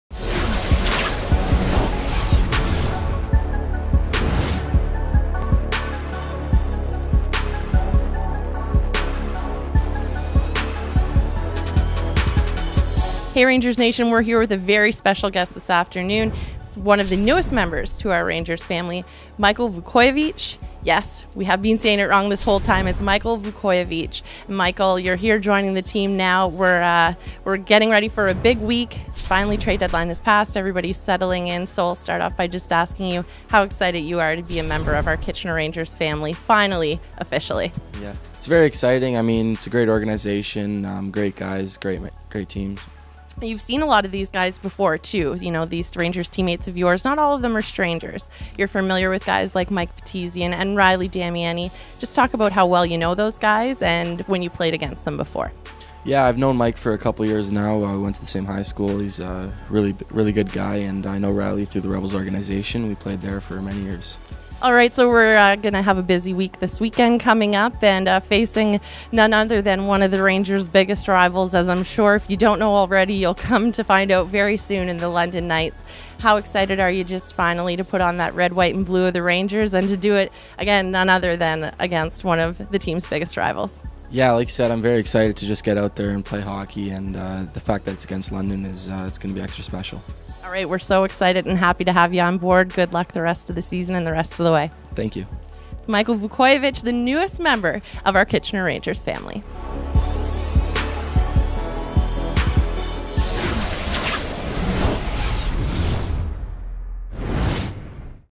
Michael Vukojevic Signing (2017-18)
Alyscia Warner interviews defenceman, Michael Vukojevic, after signing his OHL player commitment.